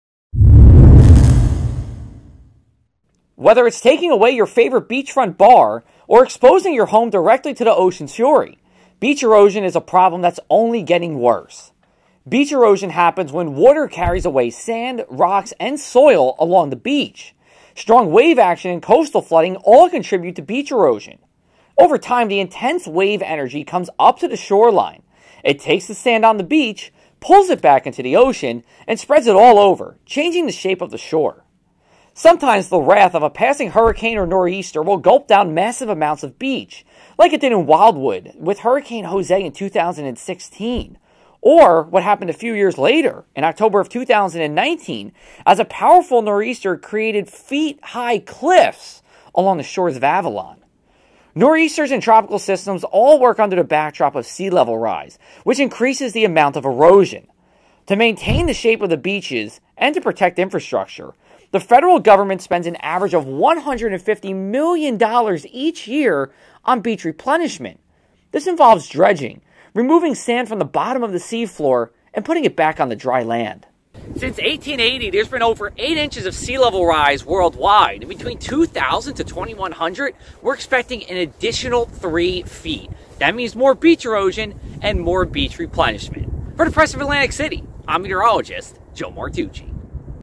What is beach erosion?
Each year, beach erosion is responsible for roughly $500 million in property and land losses in the United States, according to the National Oceanic and Atmospheric Administration. Meteorologist Joe Martucci explains why beach erosion happens and what is done to fill the beaches back up for people to enjoy.